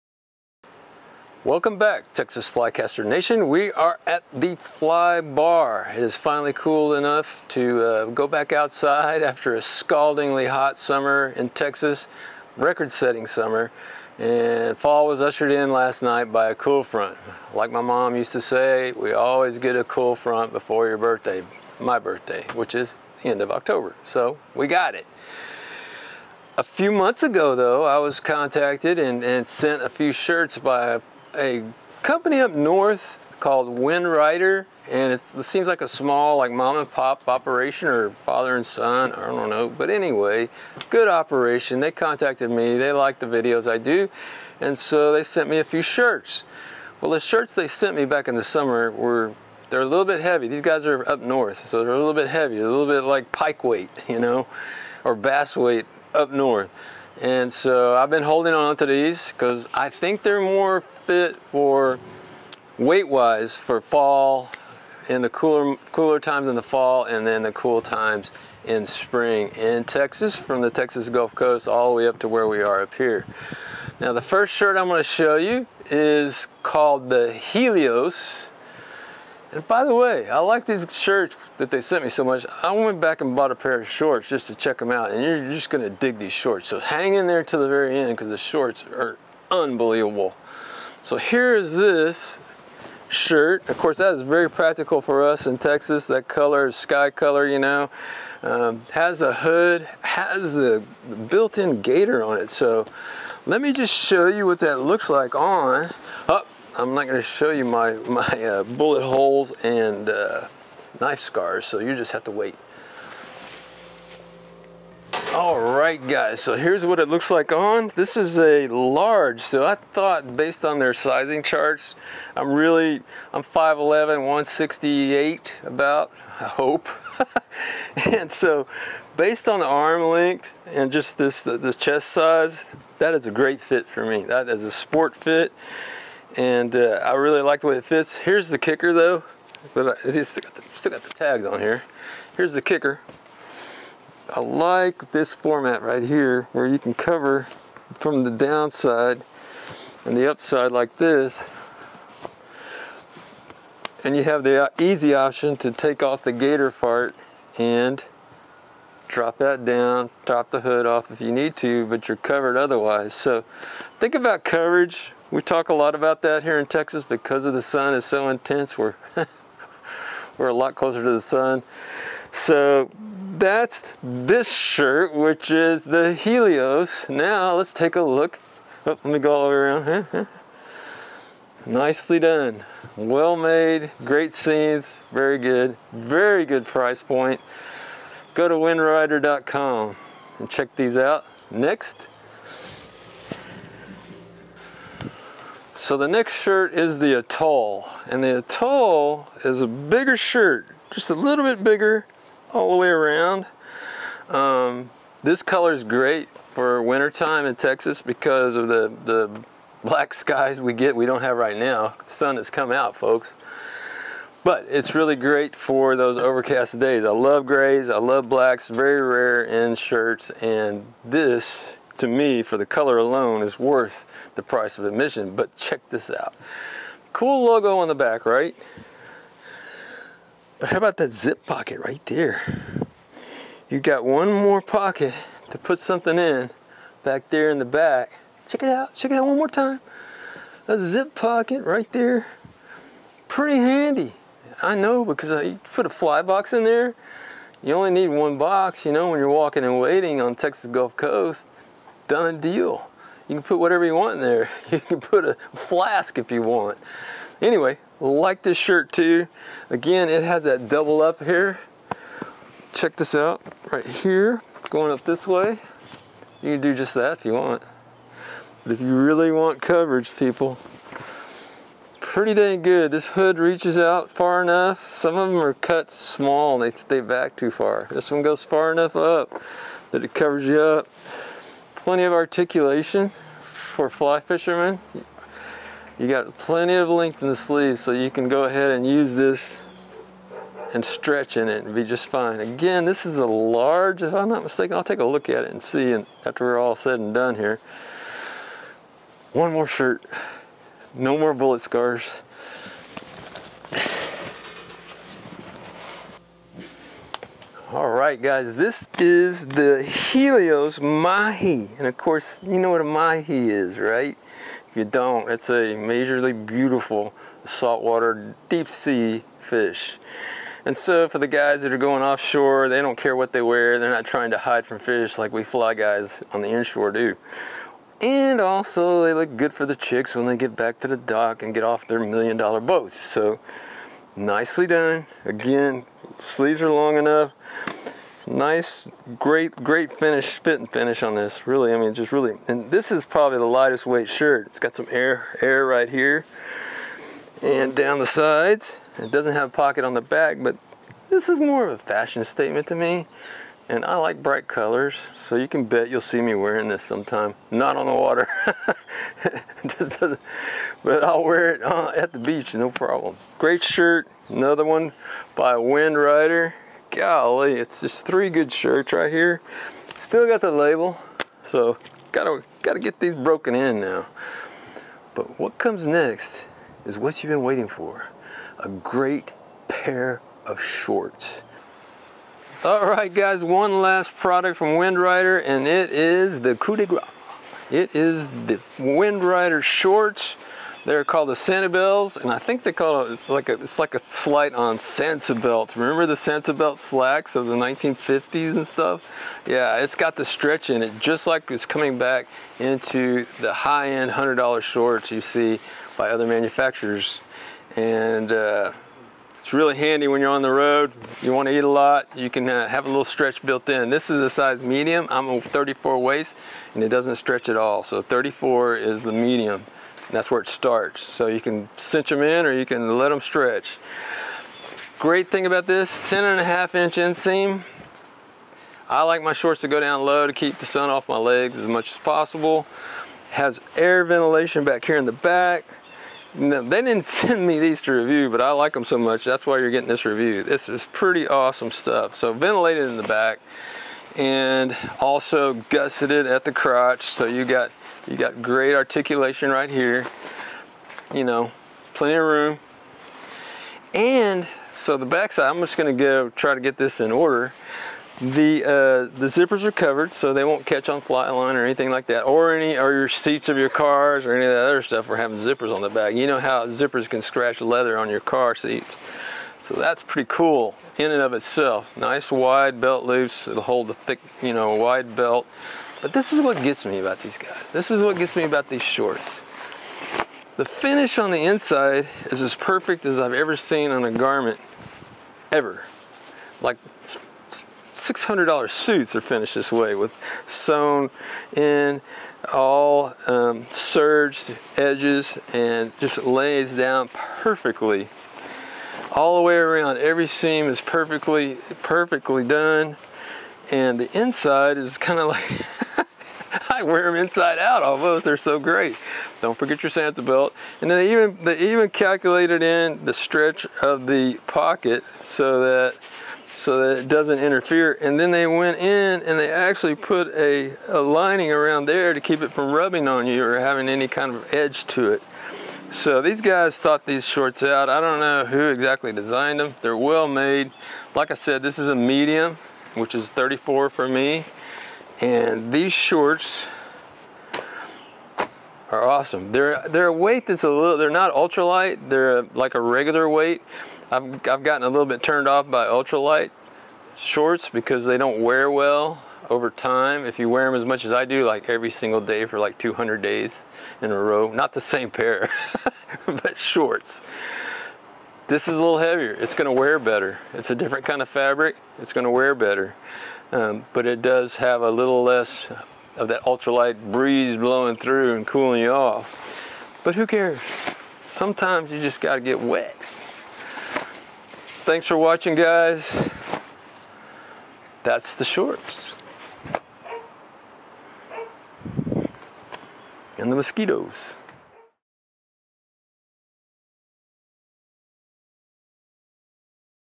Fishing Shirts and Shorts by Windrider - Review Great Fly Fishing Value and Functional Clothing
WindRider makes clothing for fishing, and sent me some to review. I like what I have seen so far, and now that it has cooled off some in Texas, I will be field testing their shirts whenever I get a chance this fall.

The company looks to be a family operation, and is based in Minnesota. That means their fabrics are a little heavier than what we wear in Texas heat. This review includes three different model shirts and one great pair of shorts - the Sanibel Hybrid Shorts. That look at the shorts is at the END of the video, so stick in there and watch.

The quality of the garments is obvious, with attention paid to the details - inside and out. And the Atoll Hoodie has a great pocket on the back that can be great for the minimalist who wants to carry as little as possible! just put a fly box in that pocket and you’re ready to walk the South Padre Island sand, or wherever you walk and wade in your neck of the woods!

It’s always fun to look at these companies when they are smaller and hungrier than the big boys. And it always makes me think about buying Howler Brothers shirts from the backseat of their car. I wonder who the next “big thing” will be in the fly world. WindRider puts function first, and it shows. They aren’t about sticking it to people either. Their pricing is amazingly good considering the market these days, and the inflation we just went through.

I will be field testing, and will be sure to followup when I have more to say about WindRider clothing!

FULL DISCLOSURE
Shirts were comped to me.
Shorts were purchased retail from the website.